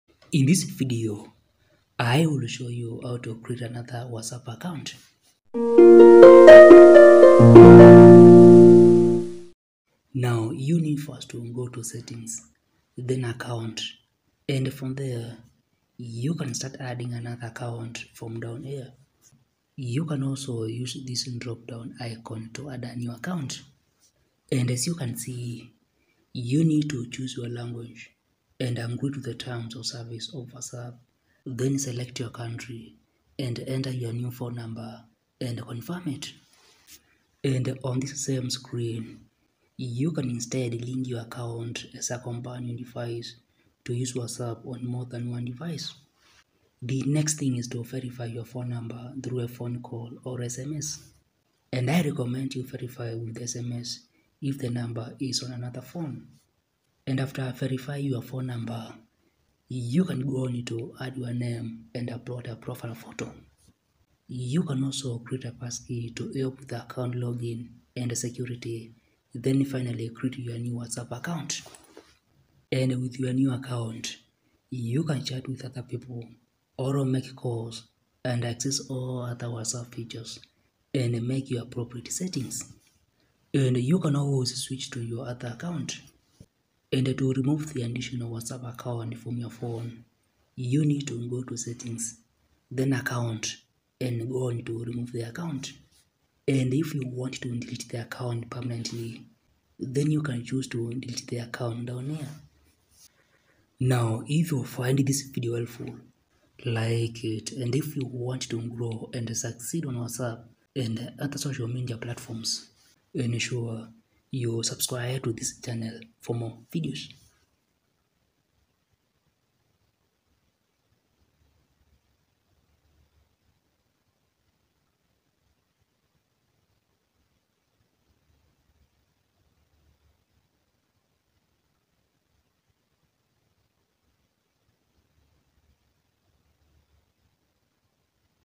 How to Create Another WhatsApp Account (NEW)
See how to create another WhatsApp account. See also how to use two accounts on WhatsApp. In addition, see how to add a new WhatsApp number on the same phone. Moreover, see how to make more than one WhatsApp account on the same device.

0:00 - Introduction
0:10 - Creating Another WhatsApp Account
1:12 - Using New WhatsApp Account
1:26 - Deleting the Additional Account
1:43 - Conclusion